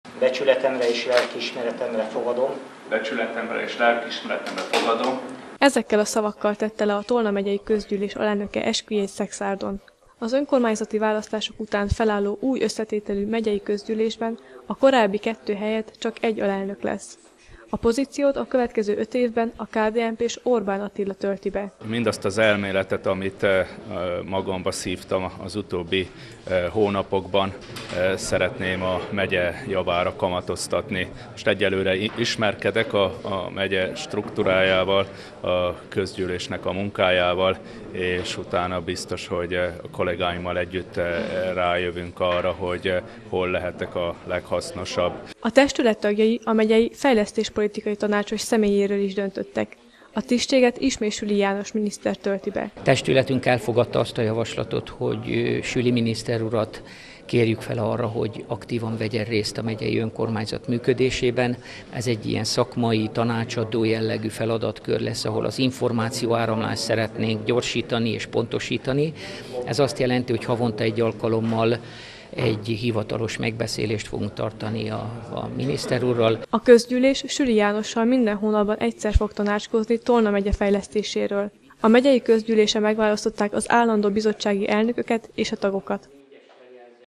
Megyei Közgyűlés - új alelnököt választott a testület 20191030
Új alelnököt választott a Tolna Megyei Közgyűlés. Az előző ciklushoz képest változás lesz, hogy 2019-től csak egy helyettese lesz az elnöknek. A titkos szavazással, egyhangúlag megválasztott Orbán Attila azt mondta, először azt szeretné megtudni, melyik területen a leghatékonyabb, utána pedig ott szeretné tapasztalatait kamatoztatni.